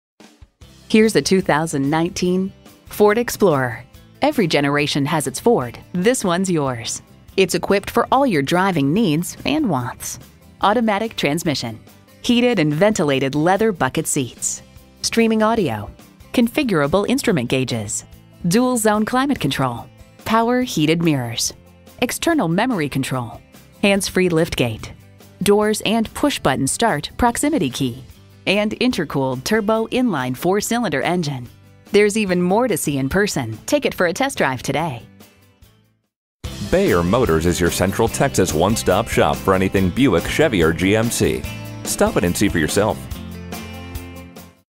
Used 2019 Ford Explorer Comanche Brownwood, TX
Year: 2019
Make: Ford
Model: Explorer
Trim: Limited
Engine: 2.3L I4 EcoBoost
Transmission: 6-Speed Automatic with Select-Shift
Color: Silver
Interior: Medium Light Stone Interior
Stock #: F21564
VIN: 1FM5K7FH2KGA95981
$3,438 off MSRP! Priced below KBB Fair Purchase Price! 19/27 City/Highway MPG Ingot Silver 2019 Ford Explorer Limited FWD 6-Speed Automatic 2.3L I4

Address:
714 E Central